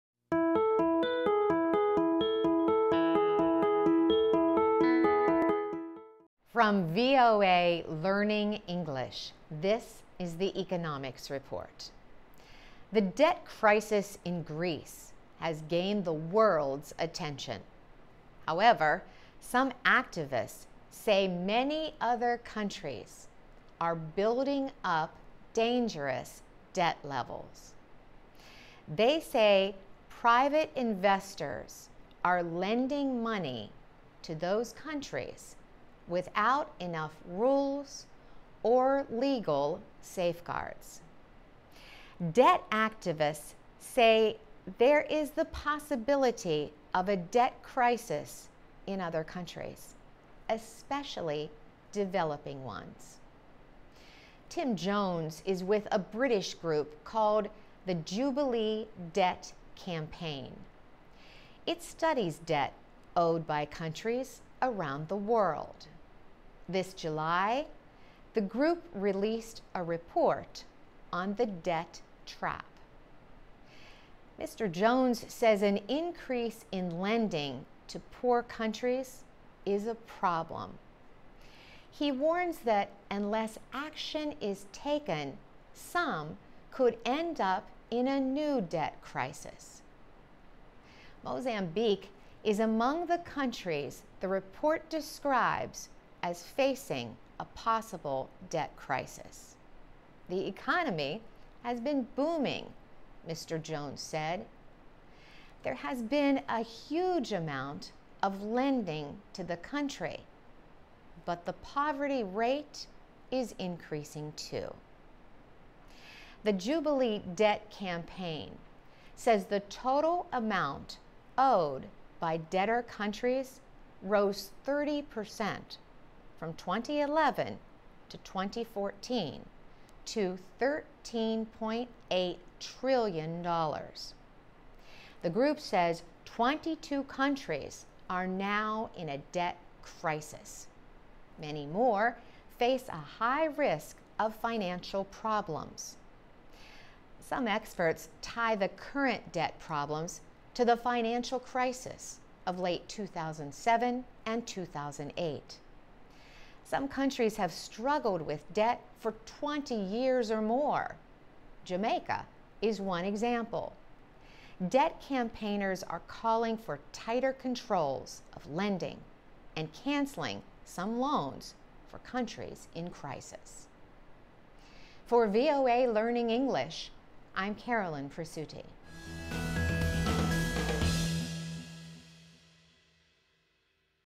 Activists Warn Greece not the Only Country Facing a Debt Crisis
The Jubilee Debt Campaign is warning of a “debt trap” for many developing nations. The group notes that net debt grew by 30 percent between 2011 and 2014. And it predicts debt levels will rise, putting many nations at risk.